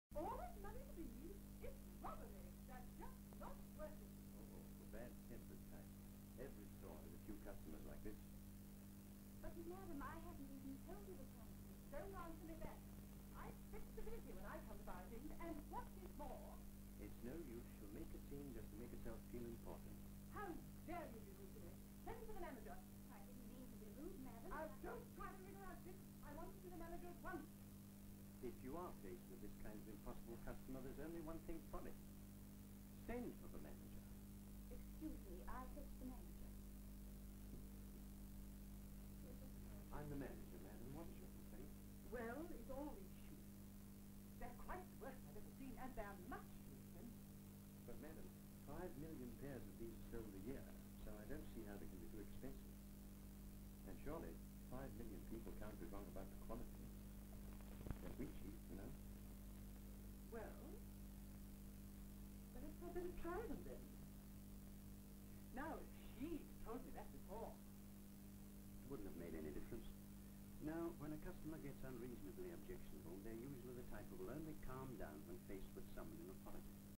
Dealing with a Bad Tempered Male Customer. Archive film 97038
Customer service in a shoe shop
Male shop assistant dealing with a bad tempered male customer